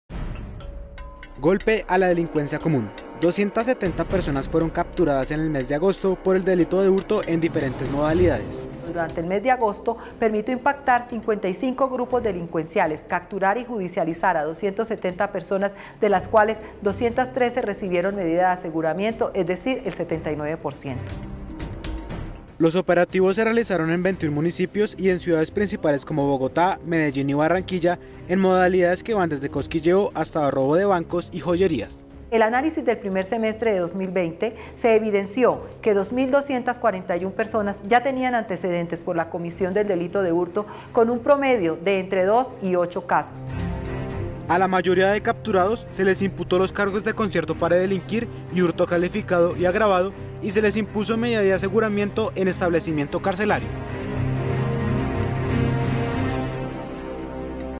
Redada contra el hurto deja 270 capturados | Noticias RPTV
BOGOTÁ (COLOMBIA). Jueves 1 de octubre de 2020 (AGENCIA DE NOTICIAS RPTV). En un operativo conjunto entre la Fiscalía General de la Nación y de la Policía Nacional, capturaron 226 personas por orden judicial y 44 en flagrancia, sindicados de cometer hurtos en todas sus modalidades.

Créditos entrevistados:
Carmen Torres - Delegada para la Seguridad Ciudadana de la Fiscalía
Equipo de la AGENCIA DE NOTICIAS RPTV:
Periodista: Julián Peña
Cámara y Edición: David Hernández
Redacción y Concepto informativo: Camilo Andrés Álvarez Pérez